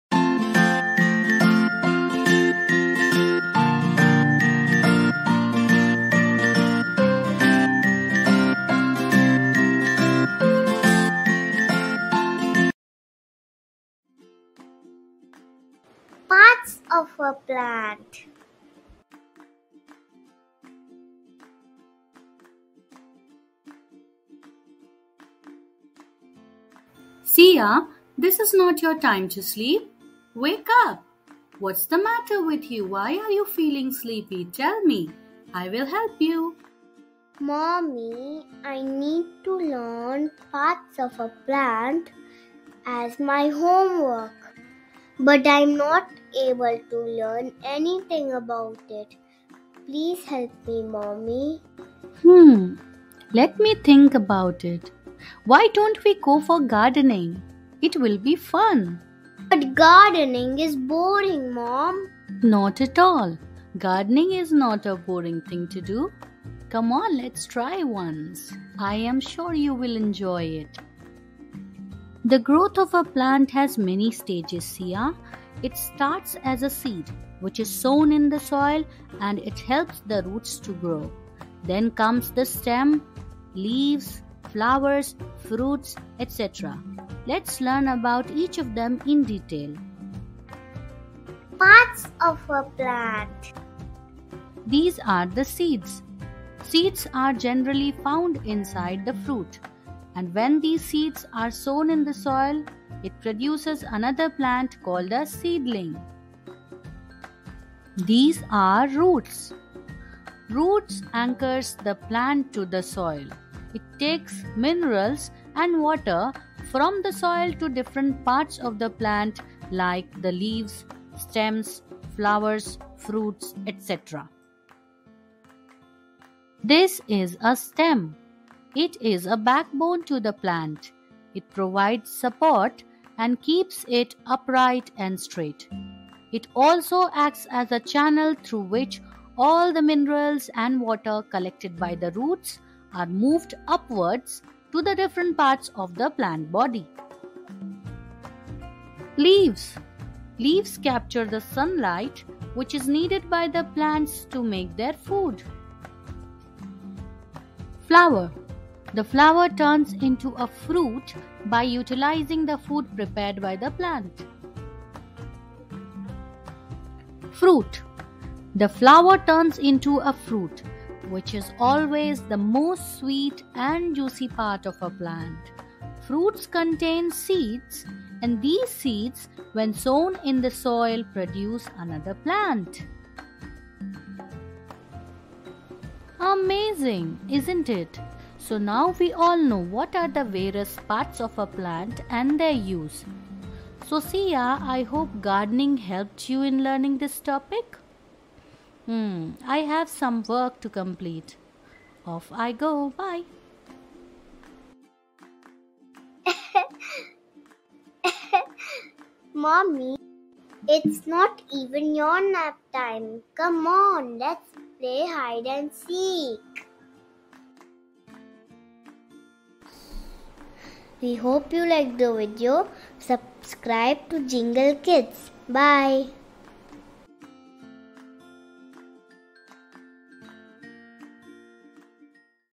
PARTS OF A PLANT - SEED, ROOT, STEM, LEAF, FLOWER, FRUIT - LEARN THE PLANT PARTS AND THEIR FUNCTIONS
Hello little ones,

Today we are going to learn about different parts of a plant body. These are seeds, roots, stems, leaves, flowers and fruits. We will learn about each one of them in detail and we are also going to discuss their functions.

Plants are an important part of human life and vegetation as well. They are very beneficial to us and give us a lot of things. We are surrounded by plants, but have you ever stopped to notice the parts of a plant? All parts of plants are important and carry out different functions. All of them come together to perform their respective functions for the healthy life of the plant. We will dig deep into the parts of plants and their functions.

1. Seeds
2. Roots
3. Stem
4. Leaf
5. Flower
6. Fruit

Let's discuss their functions:

Seed-
Seeds are generally found inside the fruit and these seeds when sown in the soil produce another plant called a seedling.

Roots-
Roots anchor the plant to the soil.
It takes minerals and water from the soil to different parts of the plant like the leaves, stems, flowers, fruits etc.

Stem-
Stem is a backbone to the plant.
It provides support and keeps it upright and straight.
It also acts as a channel through which all the minerals and water collected by the roots are moved upwards to the different parts of the plant body.

Leaves-
Leaf captures the sun light which is needed by the plants to make their food.

Flower-
The bright, colorful and the most beautiful part of the plant which you see is the flower.
The flower turns into a fruit by utilizing the food prepared by the plant.

Fruit-
The flower turns into a fruit which is always the most sweet and juicy part of a plant.
Fruits contains seeds and these seeds when sown in the soil produce another plant.

About us:
Jingle Kids is an exciting place where learning is super fun.